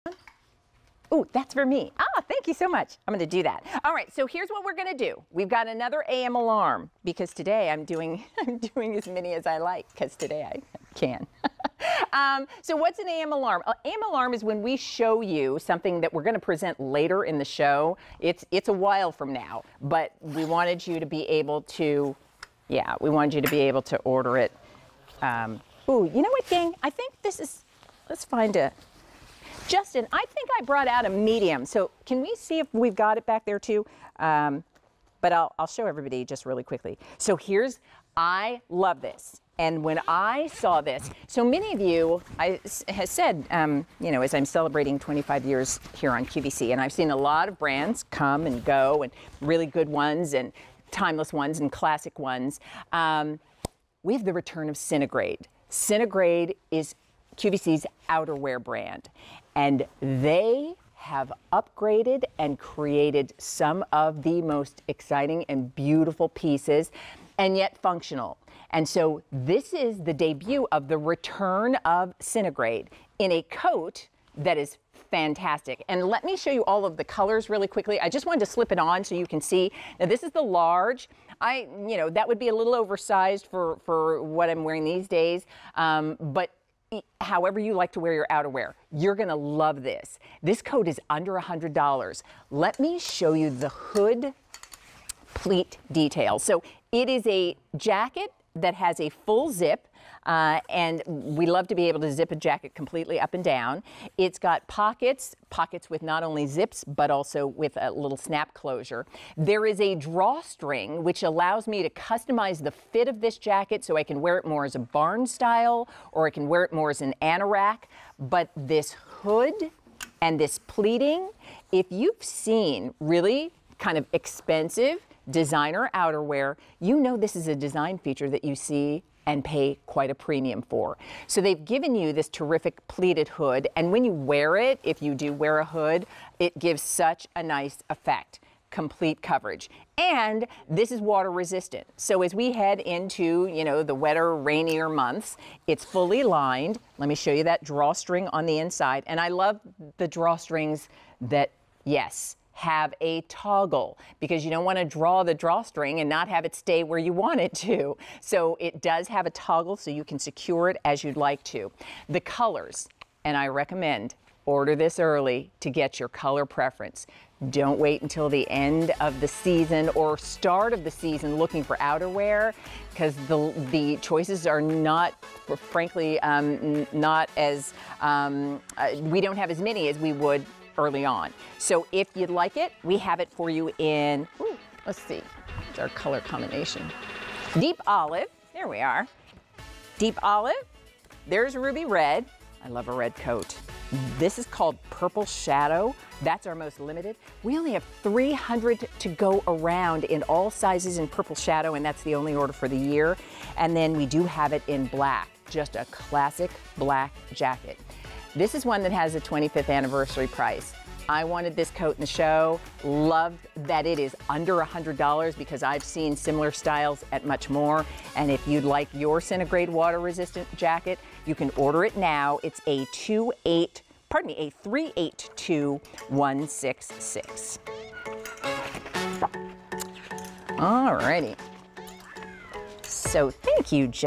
Centigrade Water Resistant Jacket with Pleated Hood Collar on QVC
Centigrade Water Resistant Jacket with Pleated Hood Collar

For those gross days when the pouring rain just won't let up, grab this water-resistant jacket off the hook and stay dry n' chic all day long. From Centigrade.